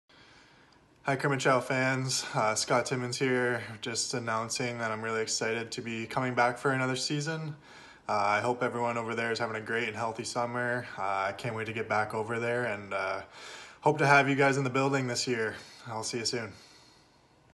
Videobotschaft Scott Timmins 💬 Stürmer bleibt ein Eispirat
Scott Timmins bleibt ein weiteres Jahr in Crimmitschau und hat nun für alle Eispiraten-Fans noch eine kurze Videobotschaft 💬🔴⚪

Feeling social?